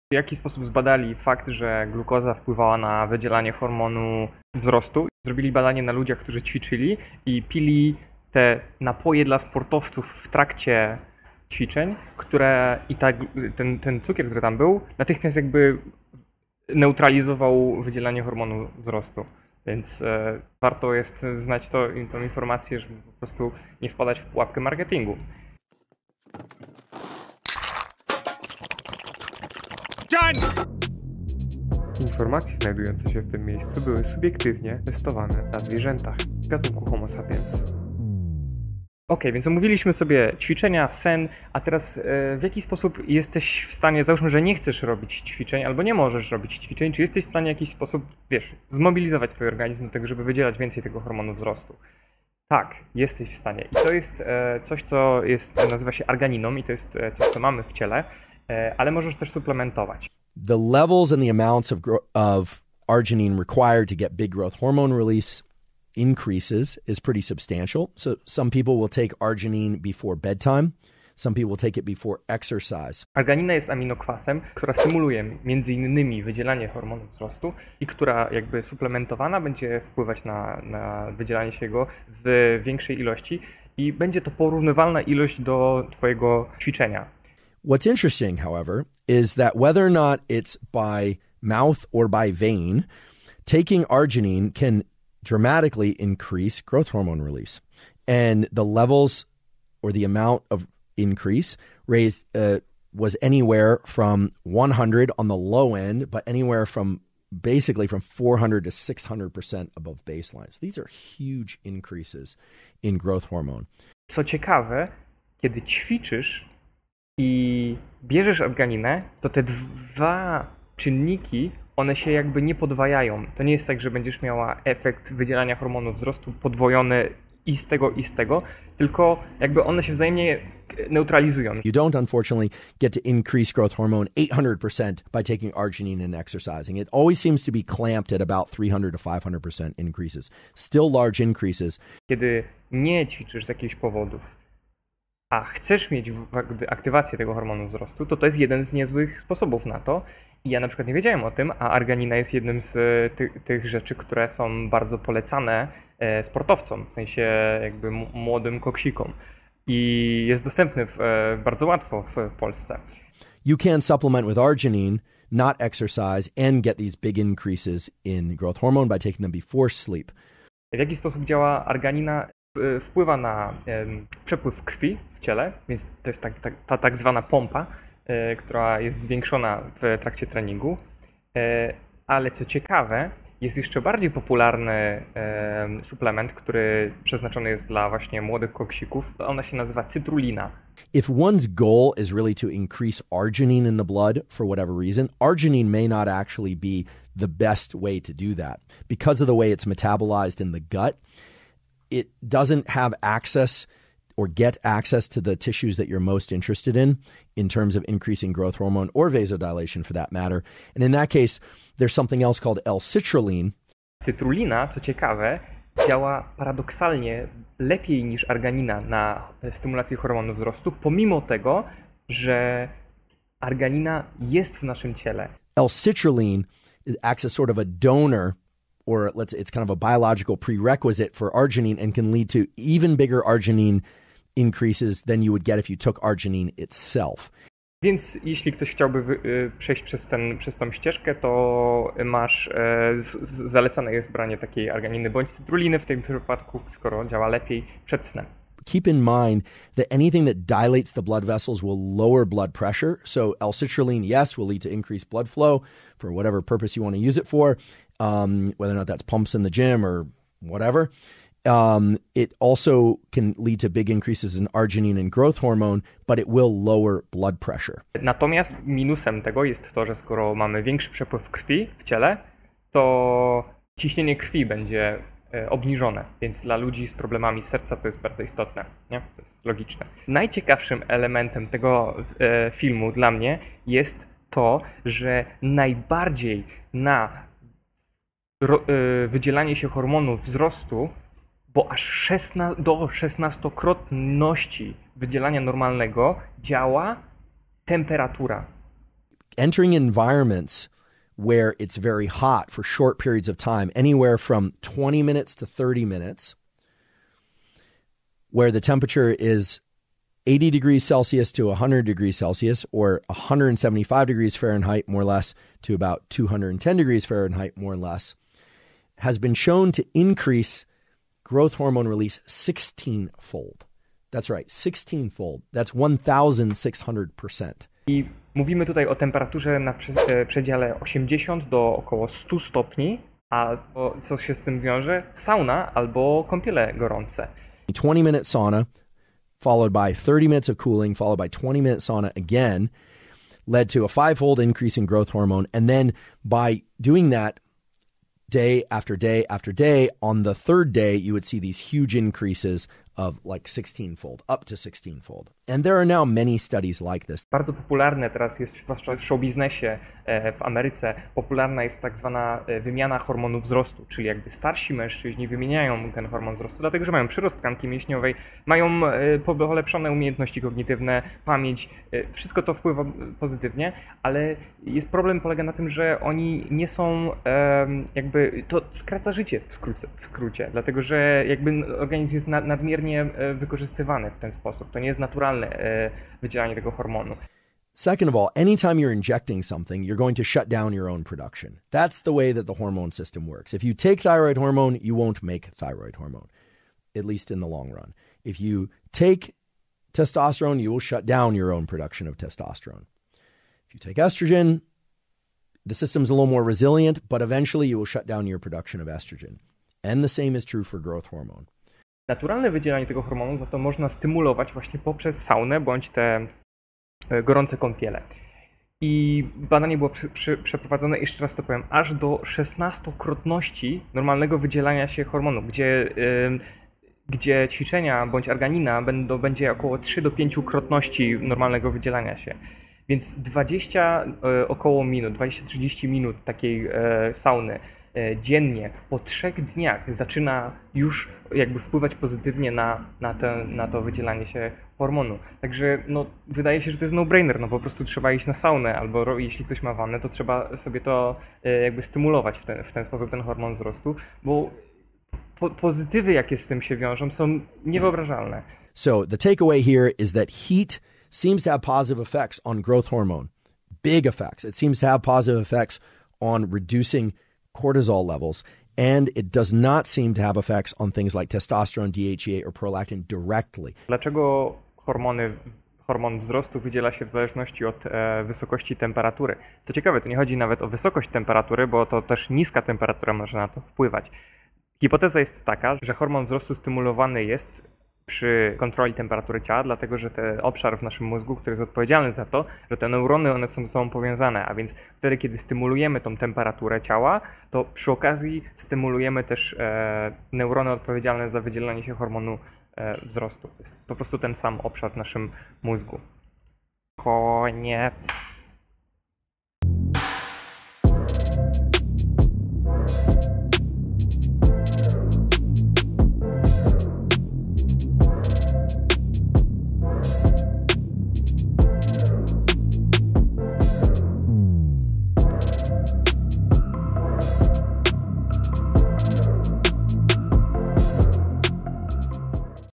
Adrew Huberman - Zdrowotne właściwości SAUNY i gorącej KĄPIELI
Zachęcam do włączenia angielskich napisów :)
Jest to druga część serii o metabolizmie, hormonie wzrostu i hormonie tarczycy z Andrew Hubermanem.

Neurobiolog Andrew Huberman mówi o działaniu hormonu tarczycy oraz hormonu wzrostu oraz o metodach przyspieszania metabolizmu.

Fragmenty animacji pochodzą z moich pozostałych filmów na kanale.